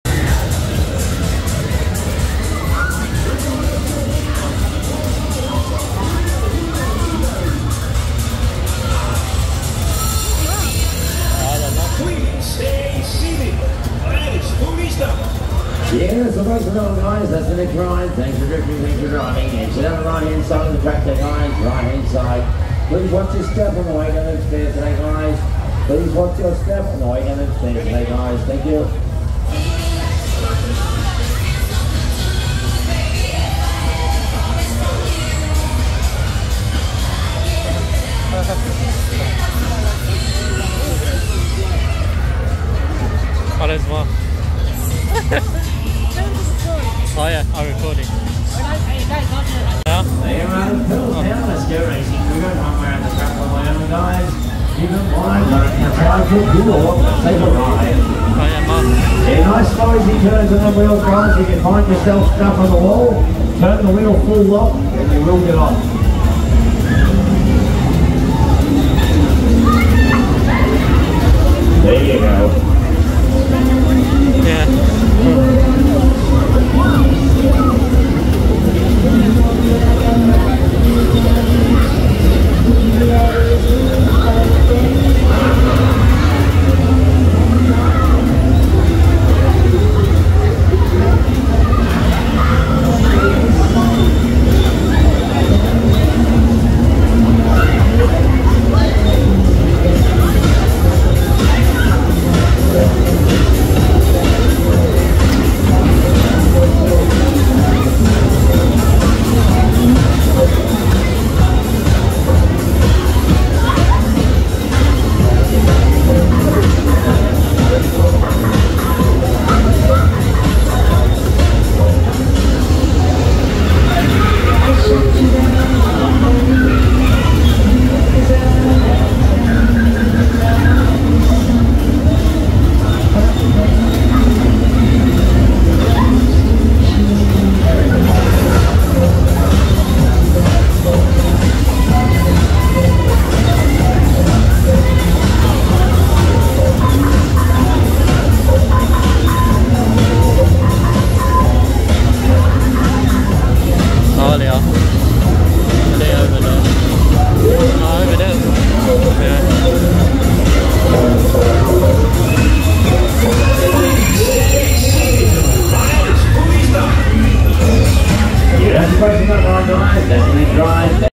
Me and Talia drove on the Bumper Cars around for the Easter Show at Olympic Park
Thanks to Danny recording this video for me.